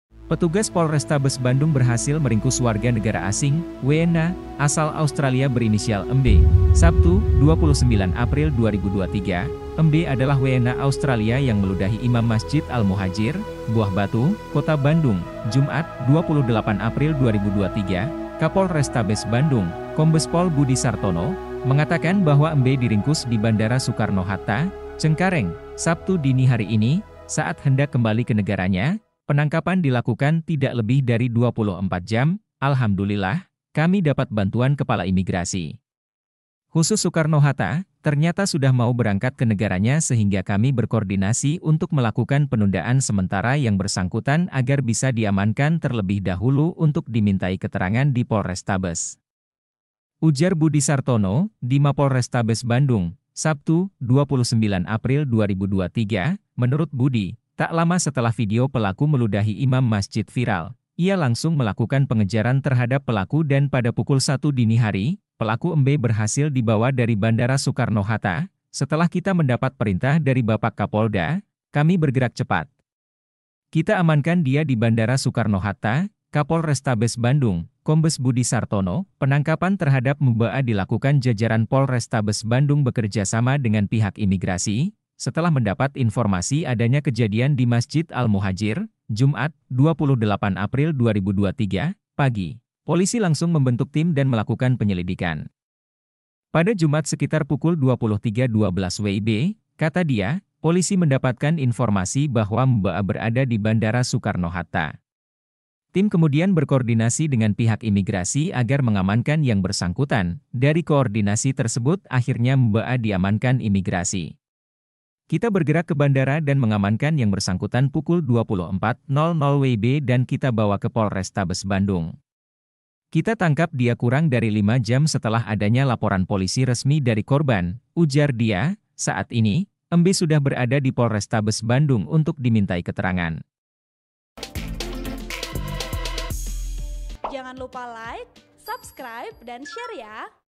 MOMEN POLISI BURU BULE YANG LUDAHI IMAM MASJID DI BANDUNG SAMPAI KE BANDARA
Artikel ini telah tayang di TribunJabar.id dengan judul WNA Australia yang Ludahi Imam Masjid di Bandung Berhasil Diringkus, Terganggu Murotal Al Quran,
Penulis: Nazmi Abdurrahman | Editor: Kemal Setia Permana |Video Editor: Uben